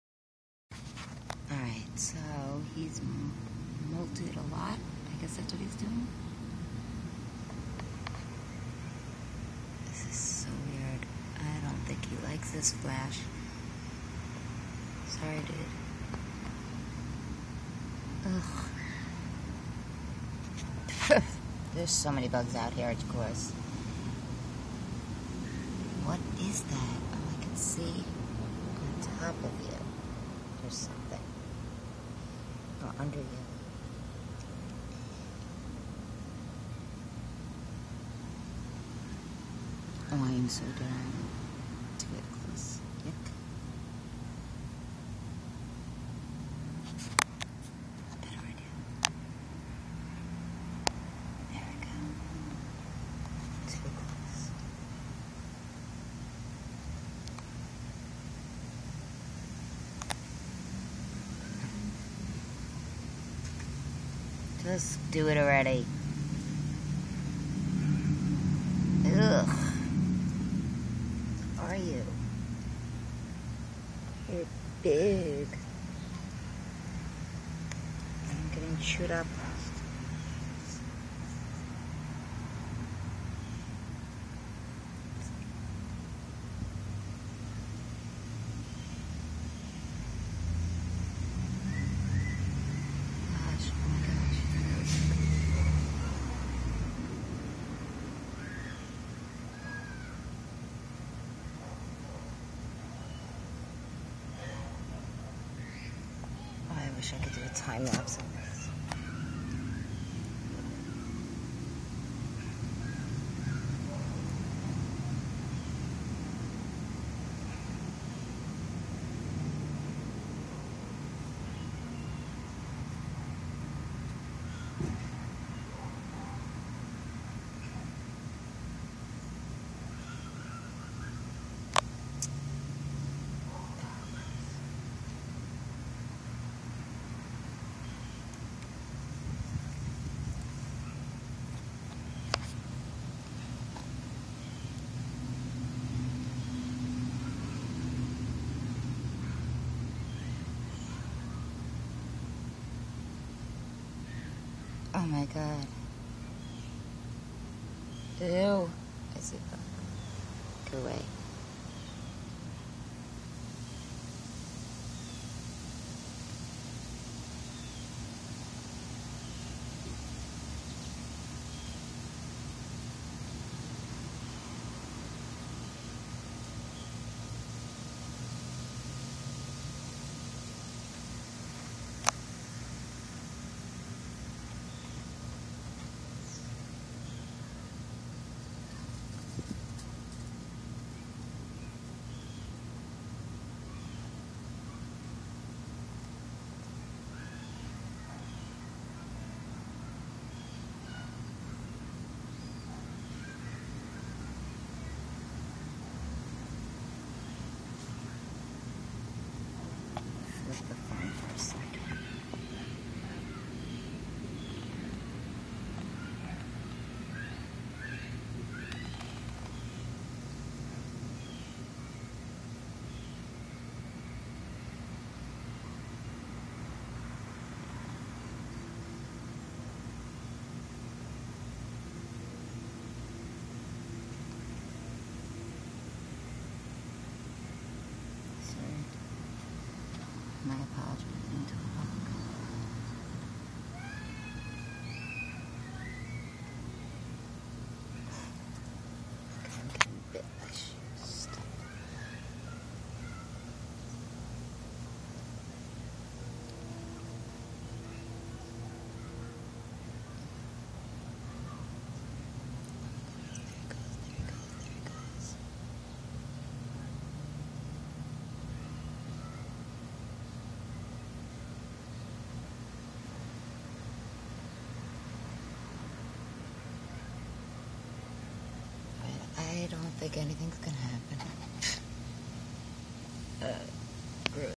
Bug: possibly molting? Can anyone identify this?
Bugs, insects, Beatles, molting, NY, New York, strange insect, Summer, identification, help